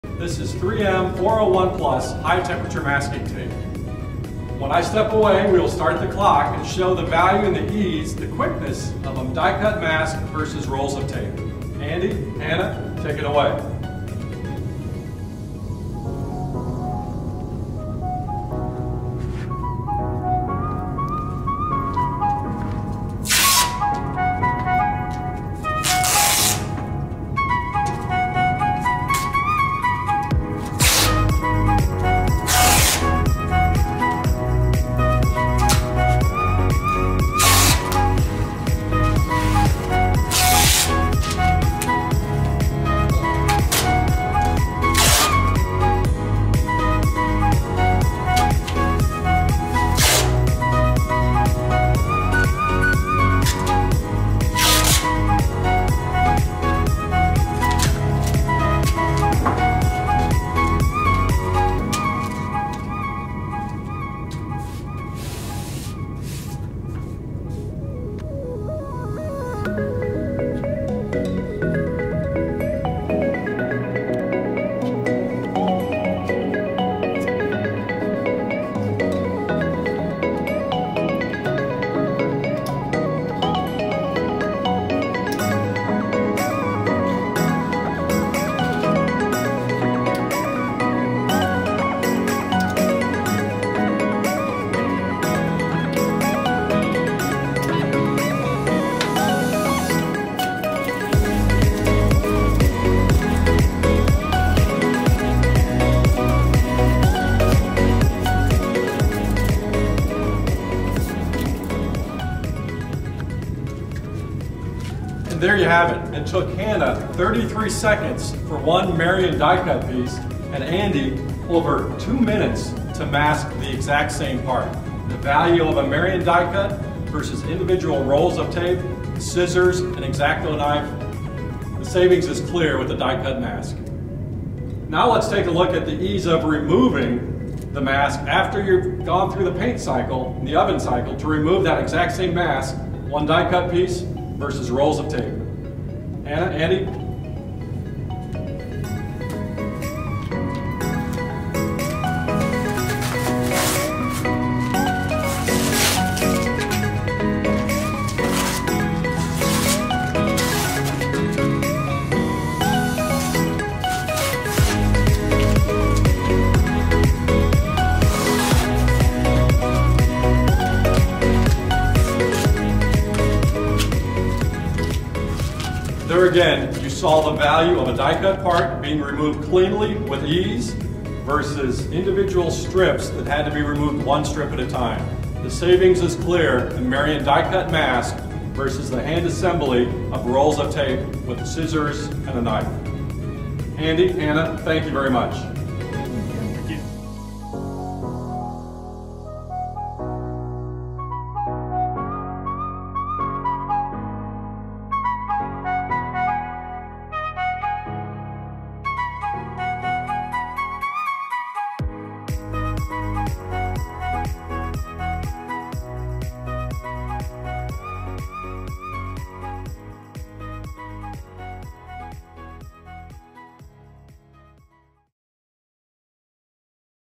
View This Demo! Die Cut Masking Tape vs. Masking Tape Strips
This video shows a side by side demonstration of the paint masking process comparing a die-cut piece of 3M 401+ masking tape versus strips of 3M 401+ masking tape. The die-cut masking piece is clearly a more simple and much faster process.